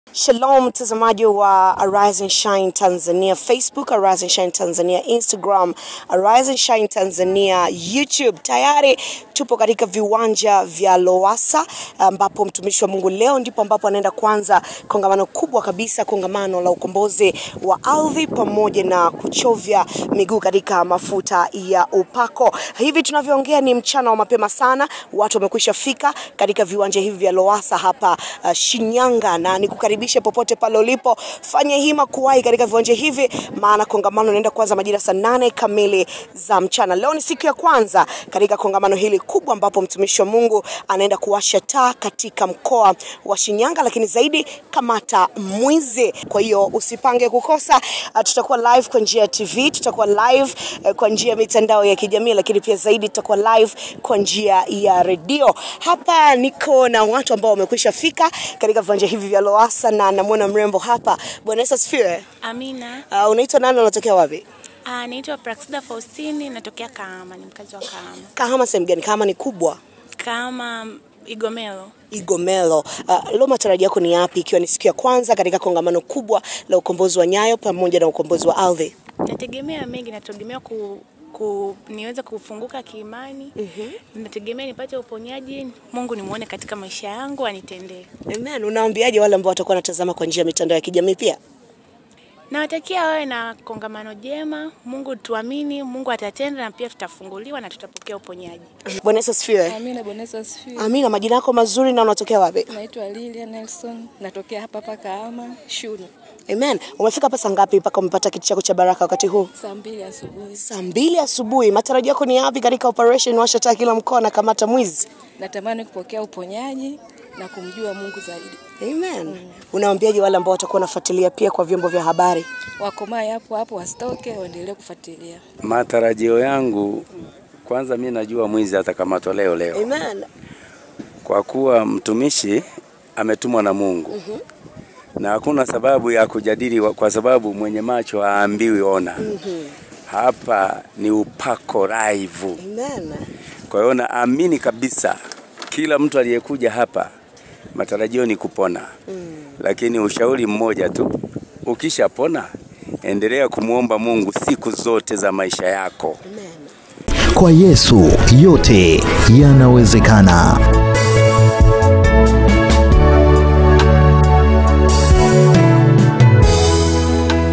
HATA MVUA INYESHE, JUA LIWAKE, MWIZI LAZIMA AKAMATWE - KAHAMA
Kwa hakika Kahama imefurika katika Viwanja vya Lowasa Mchana huu, na Watu wote wapo hapa tayari kwa kupokea Upako wa Kamata Mwizi katika Maisha yao.

Wapo wengi waliotendewa na Wapo wanaohitaji kutendewa zaidi hata sasa, na ndiyo maana Hata Mvua Inyeshe, Jua liwake Mwizi lazima Akamatwe katika Jina la Yesu.

DAY ONE KAHAMA, OPERATION WASHA TAA KILA MKOA.